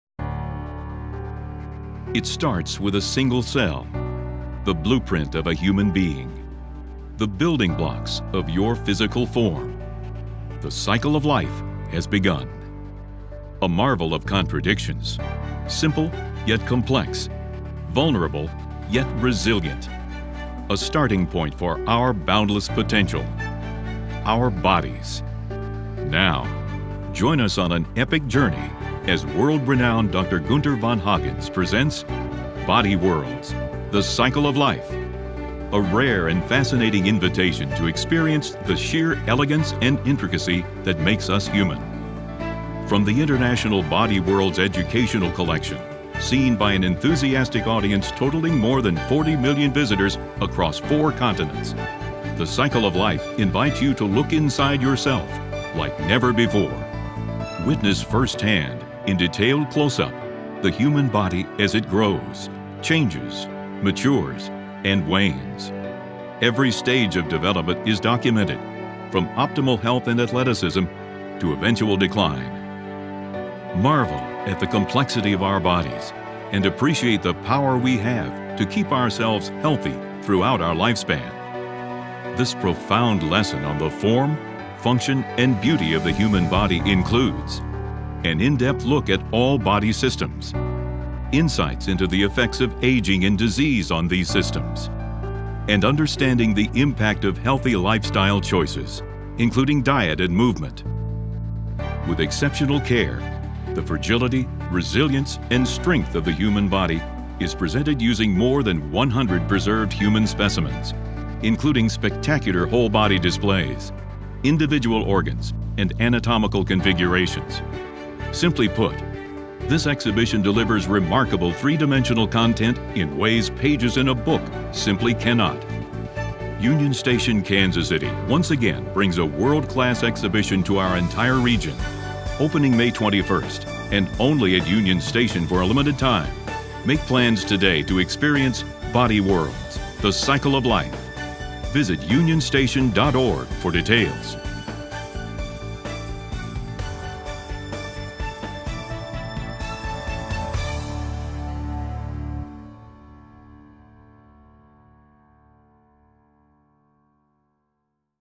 BODY WORLDS & The Cycle of Life
BODY WORLDS & The Cycle of Life is an all-new exhibition revealing the human body’s transformation through time. View more than 100  human specimens preserved through the remarkable process of Plastination, and see firsthand how the choices we make each day impact our incredible bodies – at work, at play, in health and in illness. Coming to Union Station Kansas City May 21, 2016